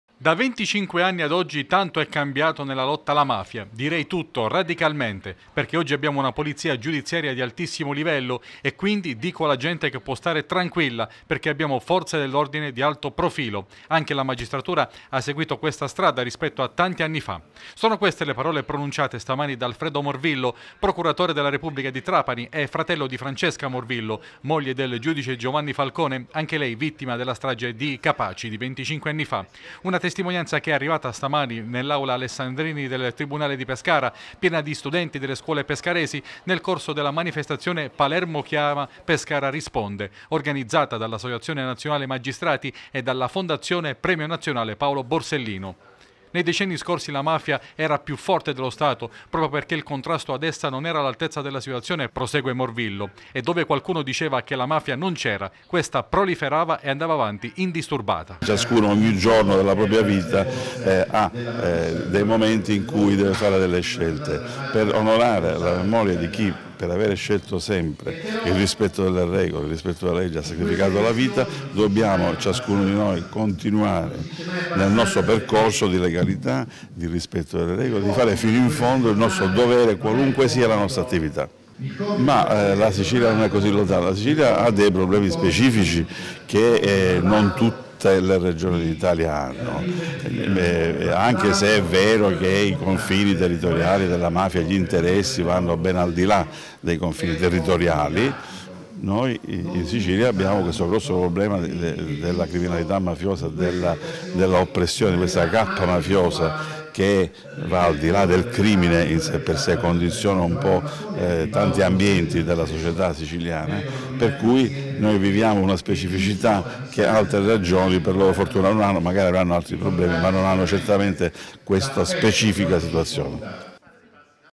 Palermo chiama Pescara per ricordare Falcone e Borsellino
Questa mattina, nell'aula Alessandrini del Tribunale di Pescara, è stata celebrata la cerimonia di "Palermo chiama Pescara risponde". Alla mattinata ha partecipato anche Alfredo Morvillo, fratello di Francesca, la compagna del giudice Falcone morta nella strage di Capaci assieme agli uomini della scorta